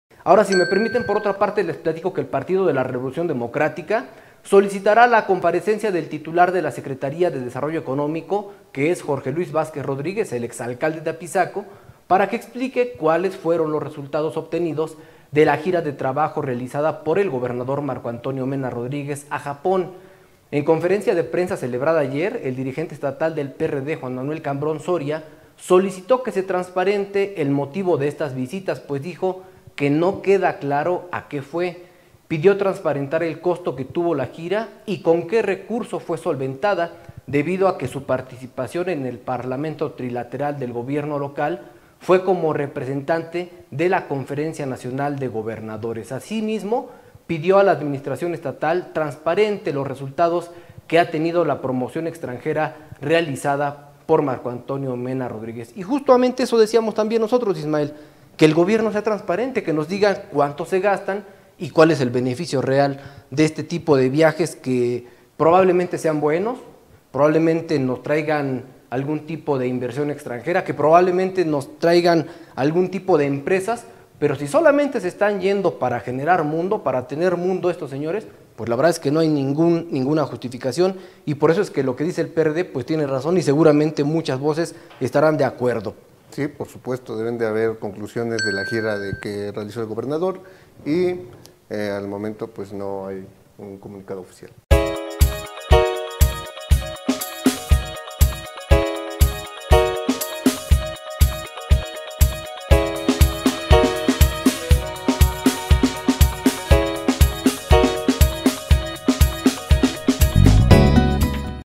Pide PRD cuentas a Ejecutivo sobre los viajes al extranjero
Más información en:

Facebook: Martín Rodríguez Hernández
Twitter: Martín Rodríguez Hernández
Instagram: Martín Rodríguez Hernández
mrh_enlinea